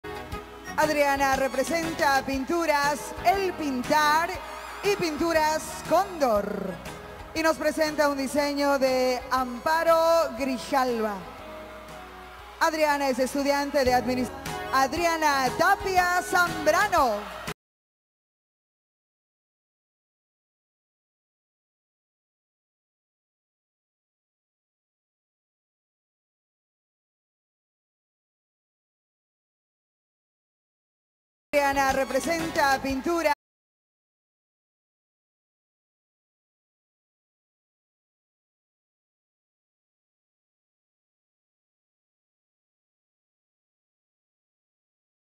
Adriana Tapia FALLS during Miss Santo Domingo 2019 Evening Gown Competition
During the Evening Gown competition of the Miss Santo Domingo 2019 beauty pageant ("Elección Reina Santo Domingo 2019"), held on June 15, 2019, candidate n°4, Adriana Tapia, was wearing a beautiful silver dress designed by Amparo Grijalva paired with very high heels - however, it proved a little too much for her as she lost her balance and completely fell down on the podium, losing all grace and elegance! Slow motion and close-up view from 0:33.